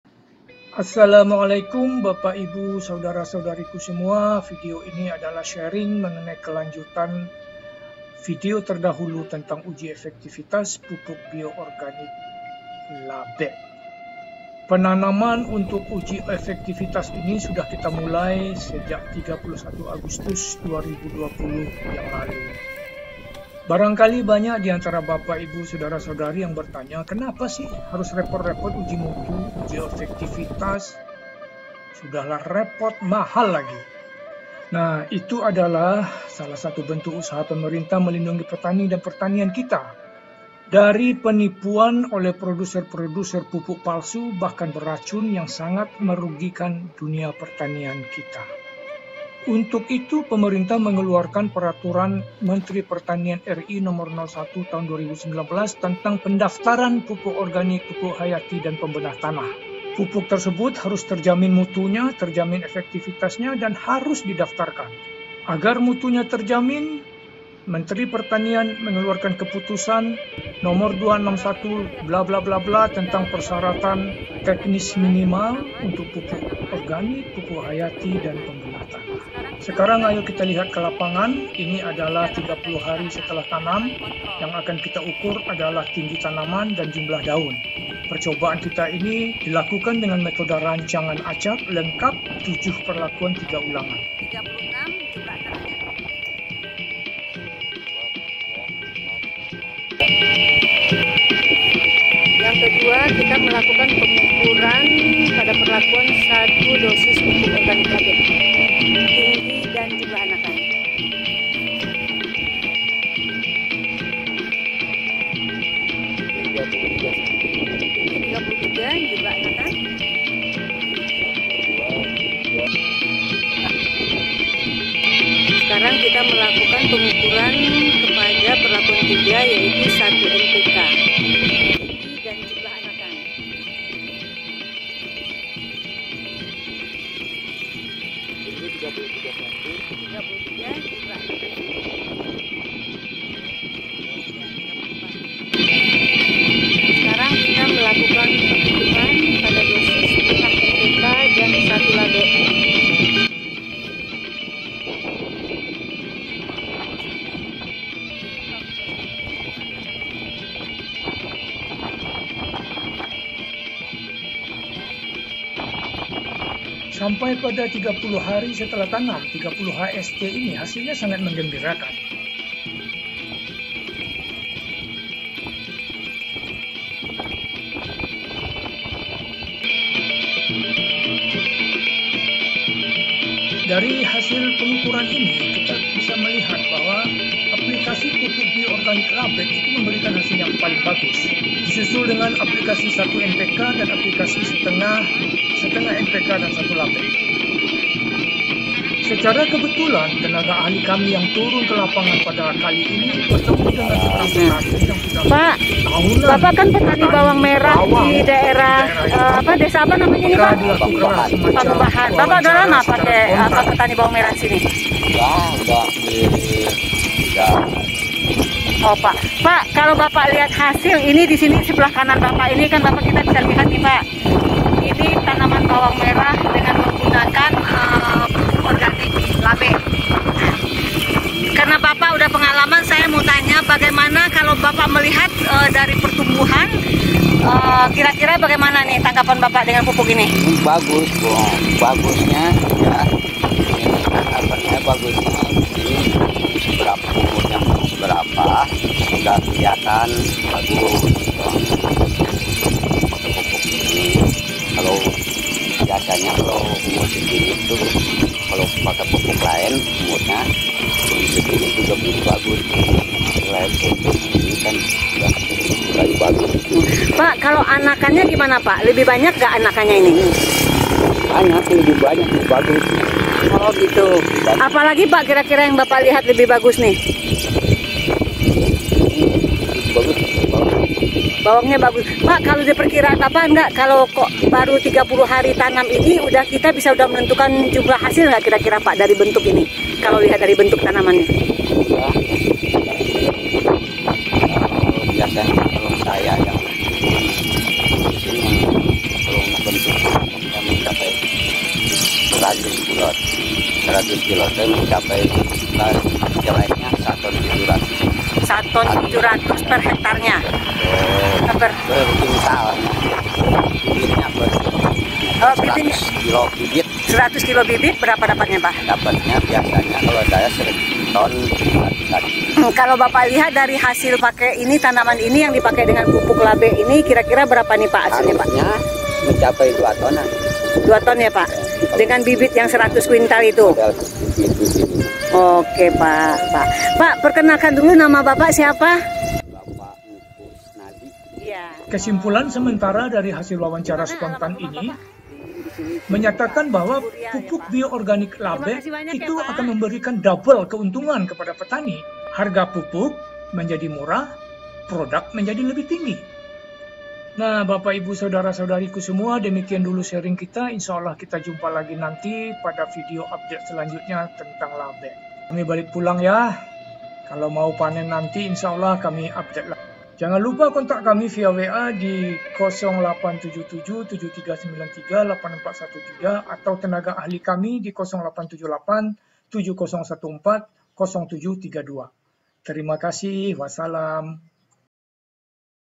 LABEK SERIE 3 PROGRESS UJI EFFEKTIFITAS 30 HST
Assalamu'alaikum, Bpk/Ibu, Sdr/Sdri ku semua.
Pada video LABEK SERIE 3 ini, kita akan lihat progress Uji Effektifitas LABEK pada 30 HST (Hari Setelah Tanam). Uji Effektifitas Pupuk Bio-Organik LABEK ini, Alhamdulillah, memberikan hasil yg menggembirakan. Bagi kami, Uji Effektifitas ini bukan hanya sekadar mendapatkan Ijin Edar saja, tapi lebih dari itu, kita benar benar ingin mengetahui secara objektf, sebagus apa Effektifitas pupuk bio-organik LABEK ini. Kalau hanya untuk mendapatkan ijin edar, nggak perlu capek capek melakukan perjalan 12 jam p/p ke lokasi percobaan.
Tapi, kita bersyukur, kesimpulan sementara dari hasil pengukuran ini, pupuk bio-organik LABEK, insyaaAllah akan memberikan keuntungan ganda kepada petani, yaitu (1) biaya pupuk menurun, (2) produksi meningkat. Nanti setelah panen akan kita hitung secara detil. Do'akan uji effektifitas ini berjalan lancar sampai kebun percobaannya panen.
Kalau ada yg akan di diskusikan silahkan kontak kami viaWA di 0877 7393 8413  aau hubungi via HP/WA Tenaga Ahli kami di : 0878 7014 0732
Terimakasih
Wassalam